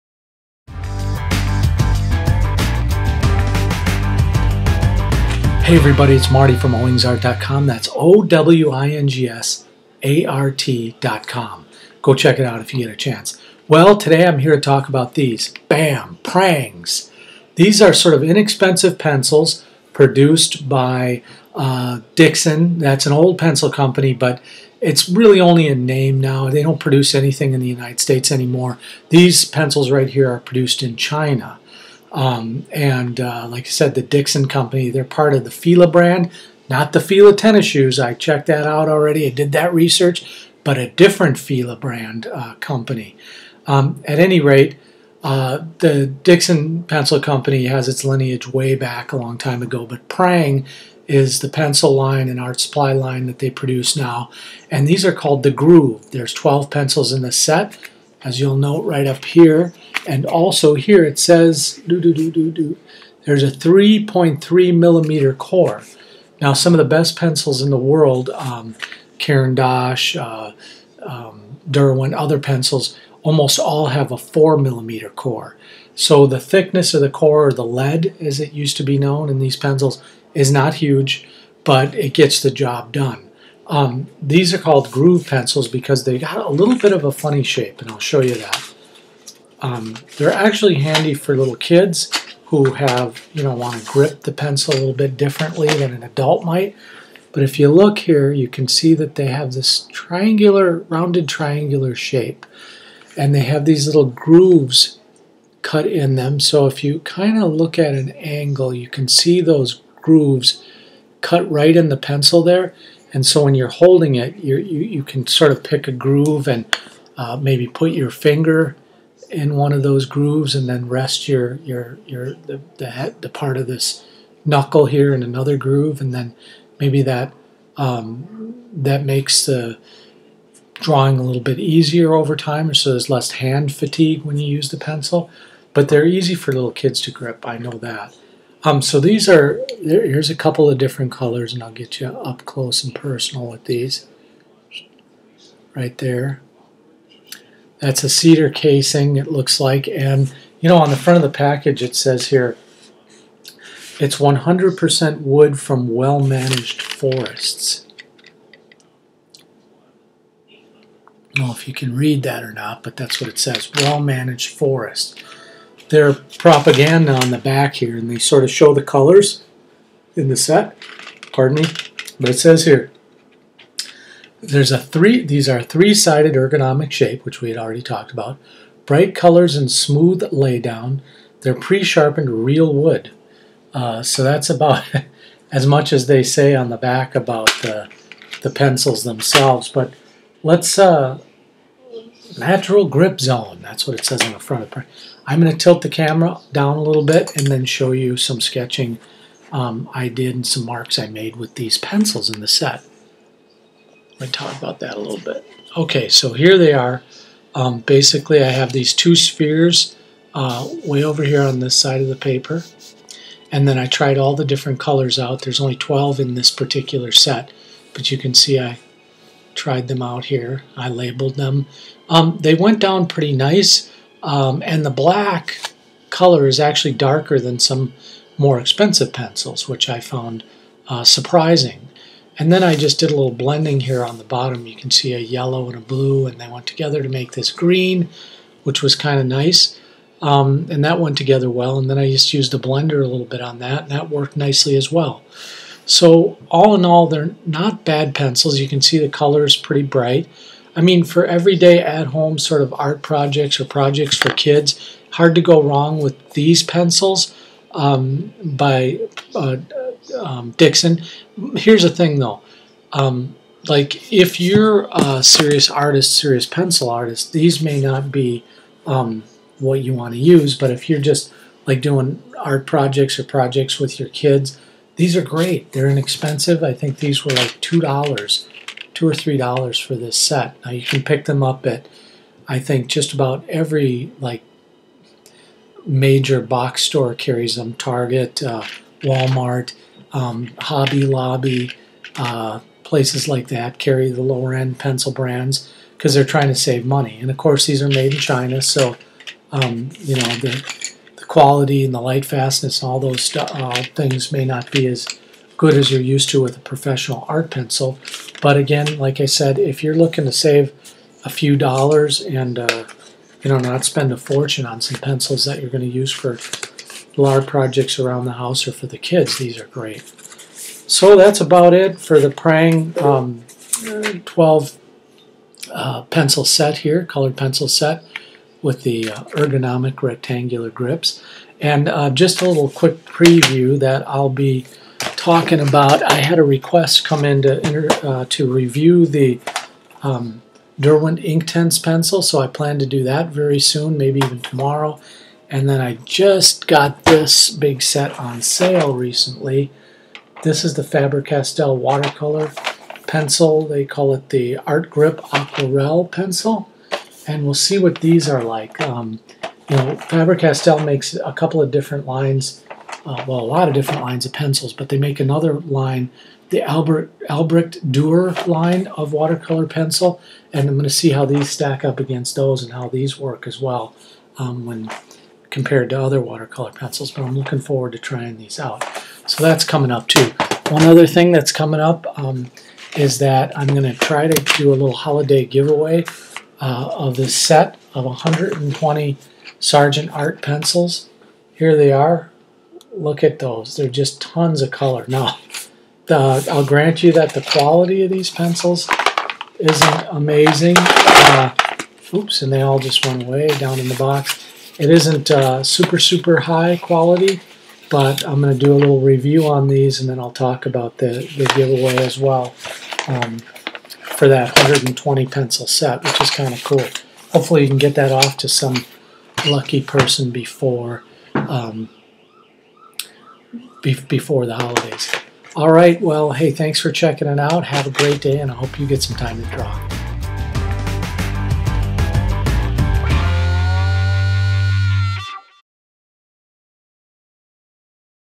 Prang Colored Pencils
A review of the inexpensive Prang colored pencil set. Made by Dixon in China this is a student grade pencil suitable for sketching or children.